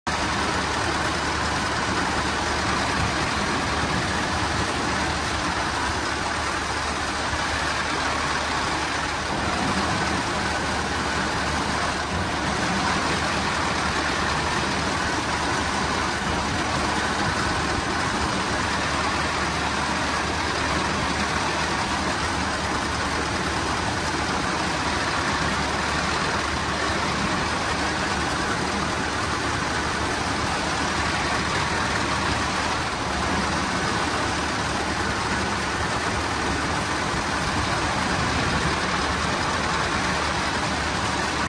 2021 ROC RT1220 For Sale
Very nice 35'5" hay merger for sale.
Freshly thru the shop and fully rebuilt!
All new teeth, cam tracks, cam track bearings, seals on axles, and all gear box oils changed.
Fixed or replaced all axle spring suspension components.
This is a clean unit, ready for the field.